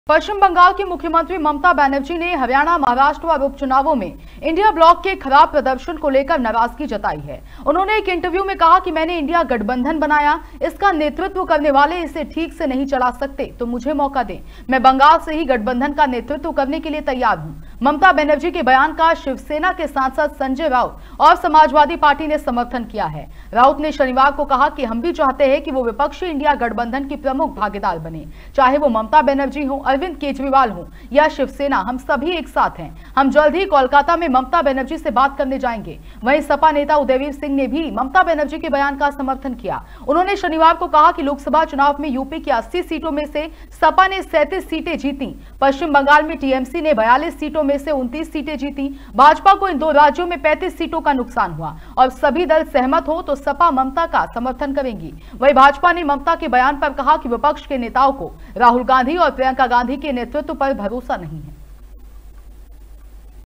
INDIA ब्लॉक के चुनावी प्रदर्शन से ममता नाराज़
"Mamata expresses displeasure over INDIA bloc's electoral performance! 🗳️⚡